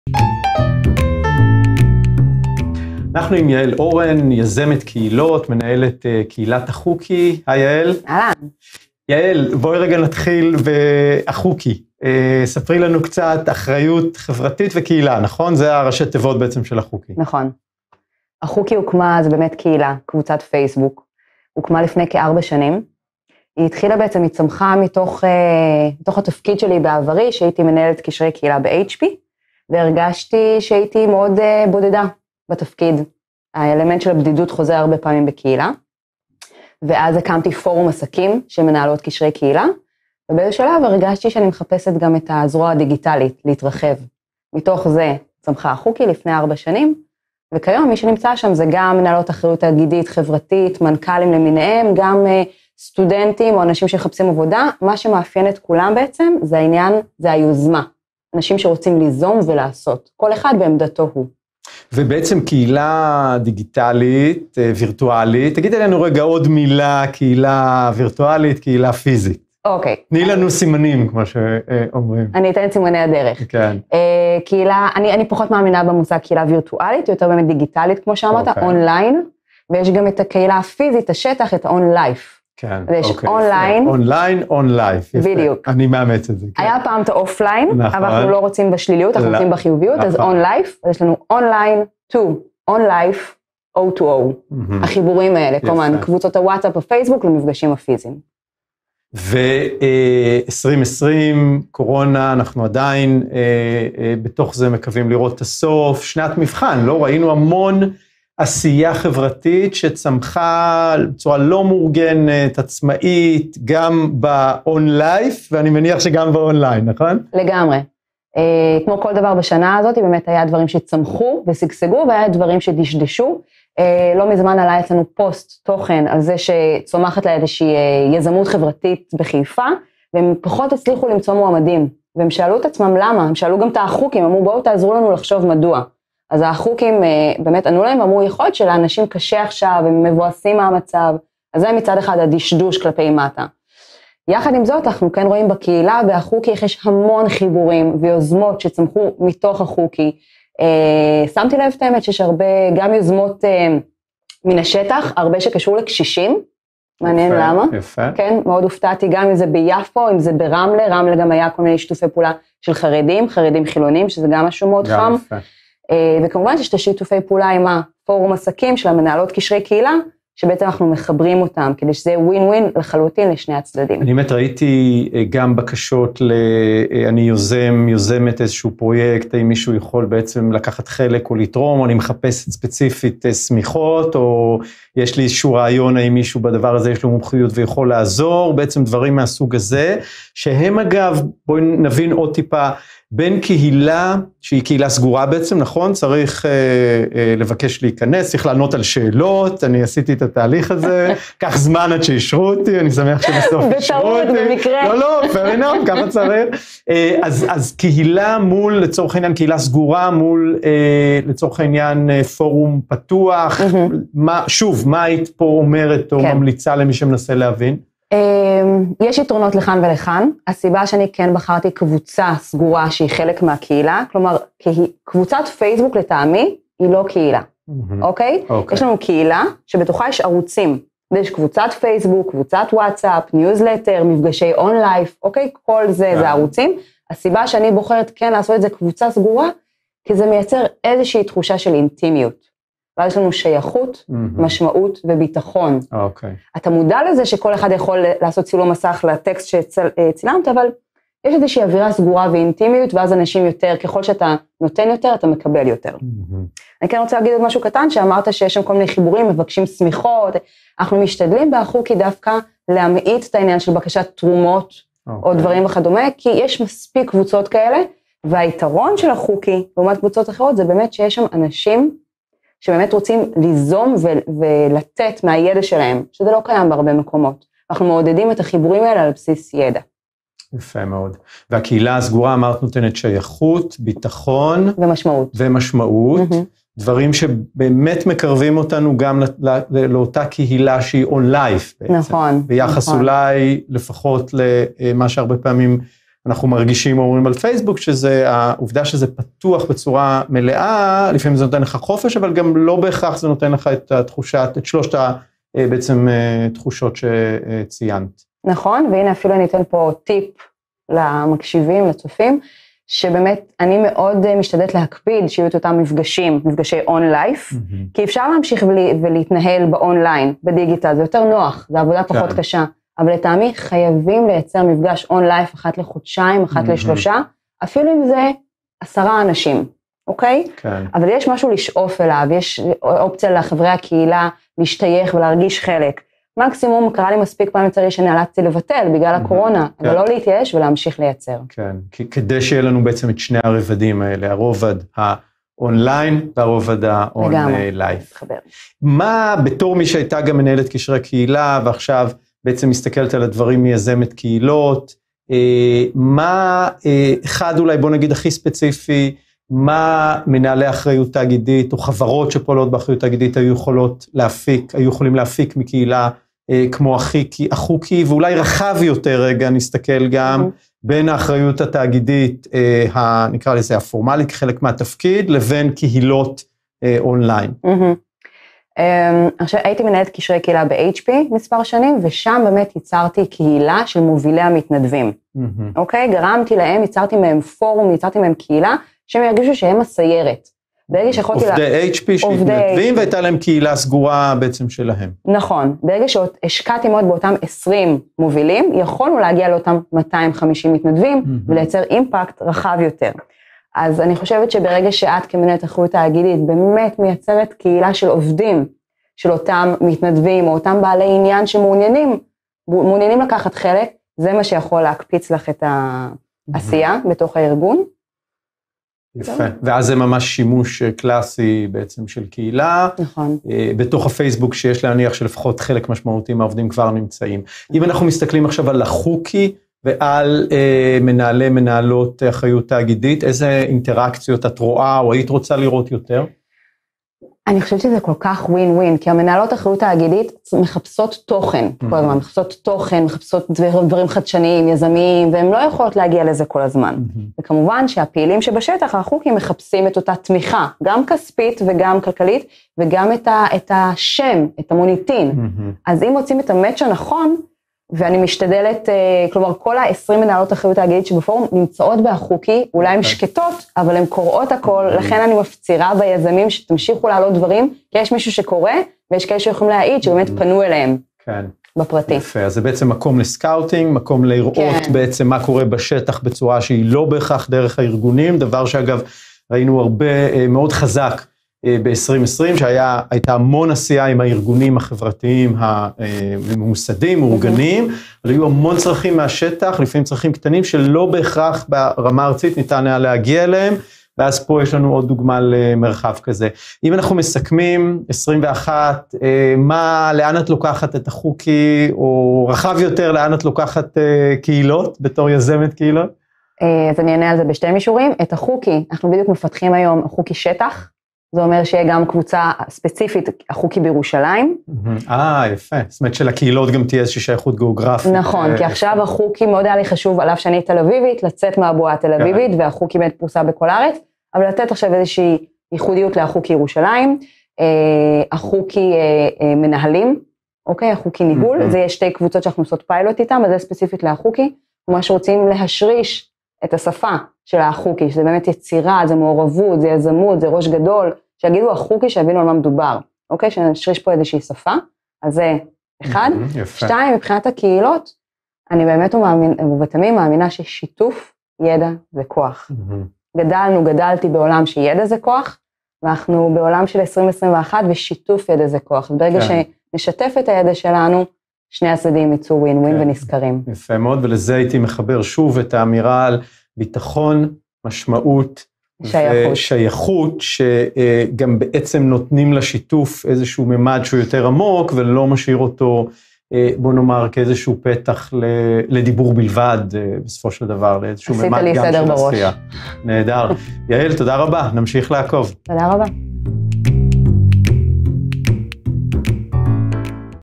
שיתוף ידע זה כוח" - קהילות ואחריות חברתית"
קהילת אחו"קי - אחריות חברתית וקהילה - נוסדה על ידי יעל אורן, מלווה ארגונים בפיתוח קהילה, לפני כארבע שנים כקהילה דיגיטלית on line.

מדובר בקבוצה סגורה שמהווה חלק מקהילה – כך נוצרת תחושת אינטימיות לצד שייכות, משמעות וביטחון. לצד הפעילות on line יעל מדגישה את החשיבות גם במפגשים הפיסיים אחת לכמה זמן, אותם היא מכנה מפגשי on life. אחת ממטרות הקבוצה זה לחבר בין יזמים ויוזמות חברתיות וסביבתיות מעניינות למנהלי אחריות חברתית ולפעילות העסקית בחברות.

יעל מסכמת בכך שבעבר הידע היה הכוח והיום שיתוף הידע הוא הכוח! וקהילה מקצועית ואינטימית שכזו יכולה לאפשר זאת.